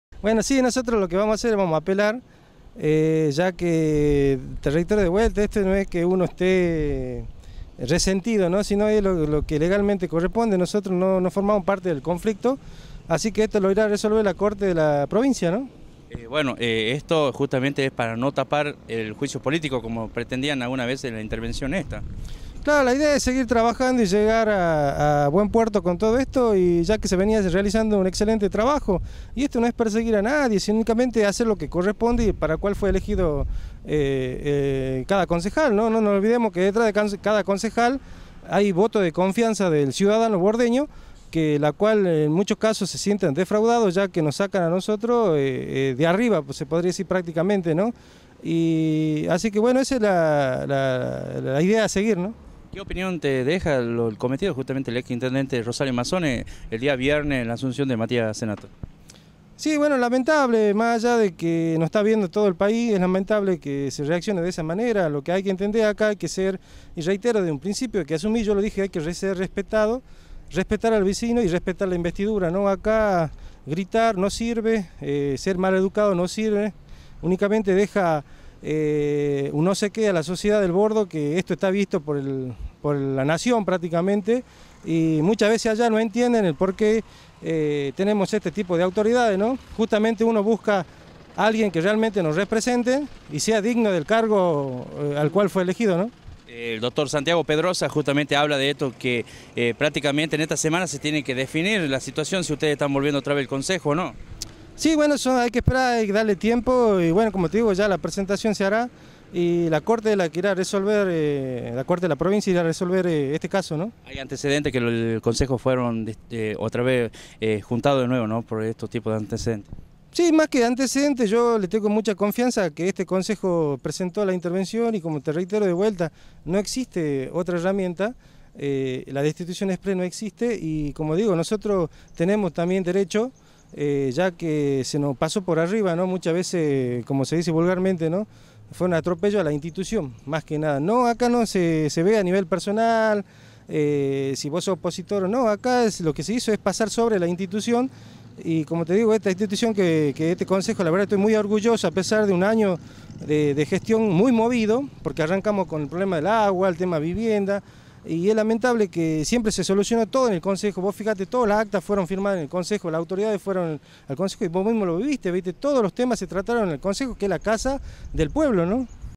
09 02 2015 EX CJAL JORGE CARI
APELARAN LA INTERVENCIÓN DEL CONCEJO DELIBERANTE DE EL BORDO